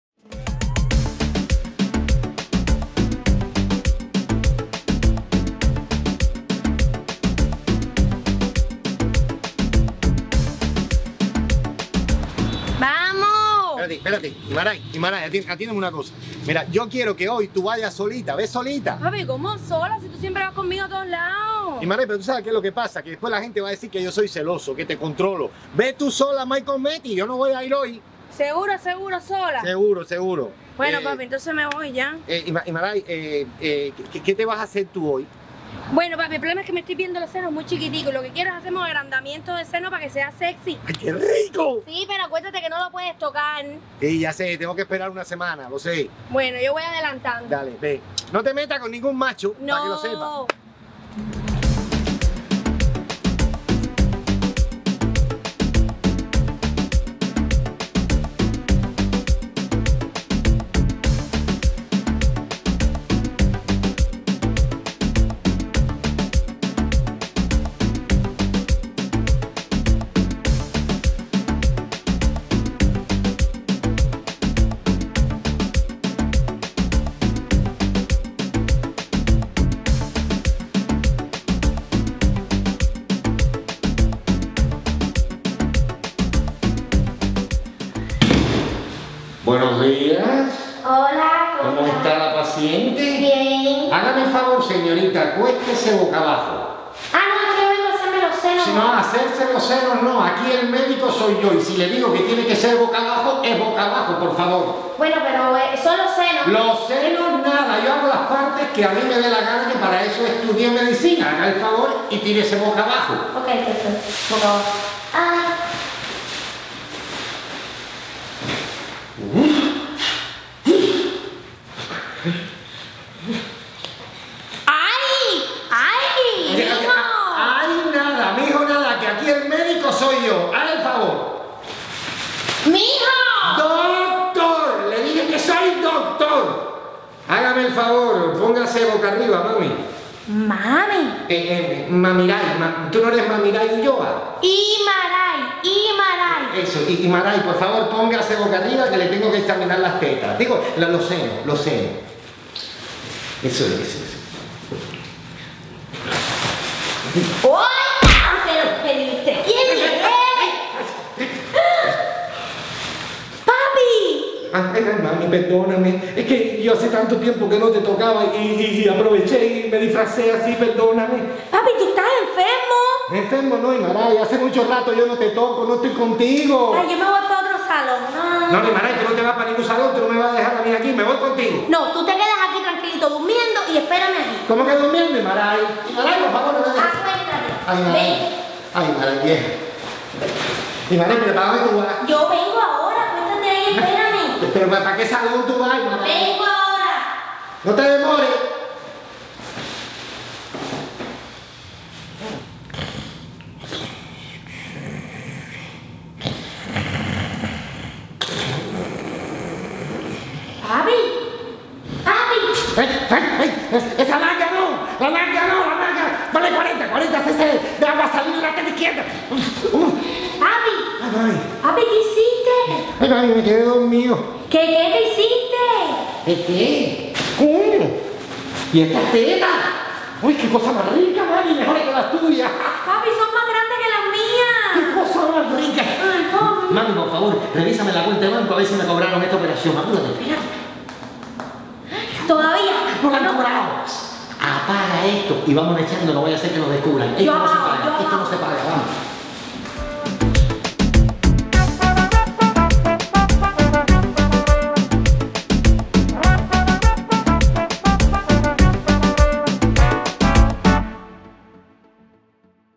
El tacaño y el disfraz
Ya este marido tacaño no sabe que hacer para estar cerca de su mujer, ahora se disfraza de cirujano.